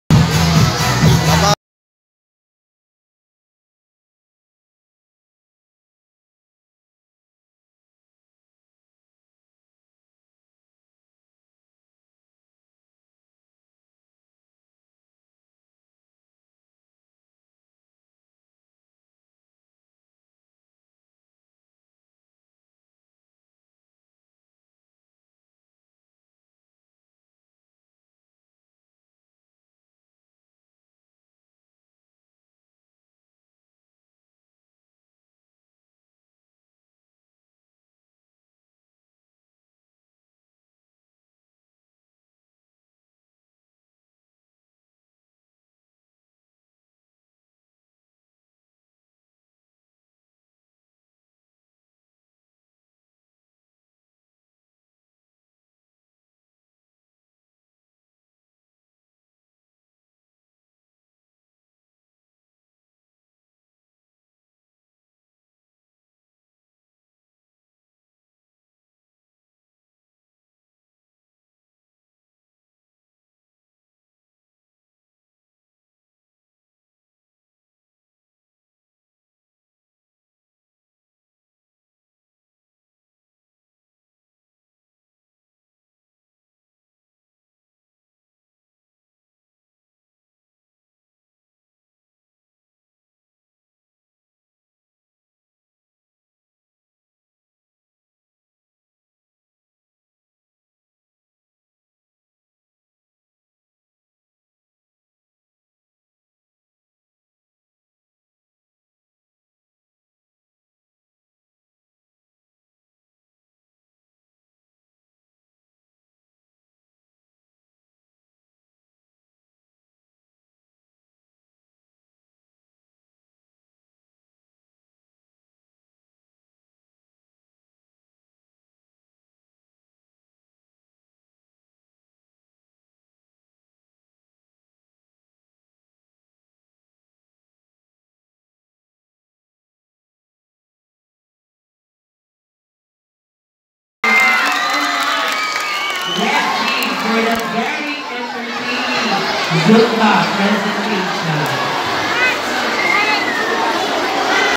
ESTANCIA ILOILO FAMILY DAY ZUMBA| Estancia Central Elementary School Grade 5
Family Day celebration ng Parents and Teachers sa Estancia Central Elementary School.
Zumba Dance Presentation.

Grade 5.